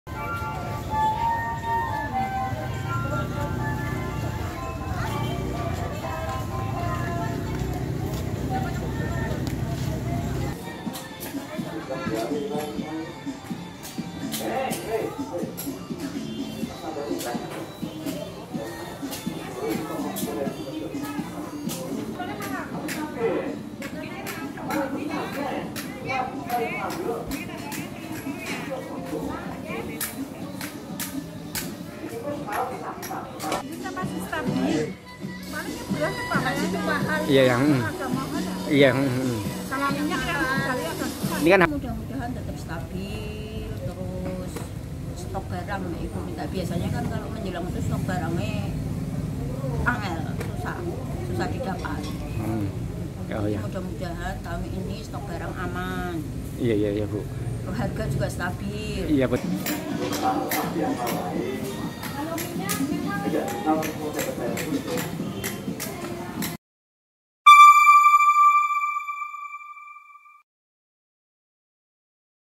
Jelang Ramadan, Harga Beras di Pemalang Alami Kenaikan
Menjelang Ramadan, harga kebutuhan pokok atau sembako di Kabupaten Pemalang, masih stabil. Hal ini diungkapkan sejumlah pedagang Pasar Kota Pemalang, Kamis (2/3/2023). Harga sembako belum mengalami kenaikan yang signifikan.

Namun, harga beras mengalami kenaikan harga. Kenaikan harga beras berkisar Rp500 sampai Rp1000 per kilogram. Sementara untuk minyak goreng masih sulit didapatkan.

Kontributor: Aryanto
(Bumper Out)
Produser : Nofellisa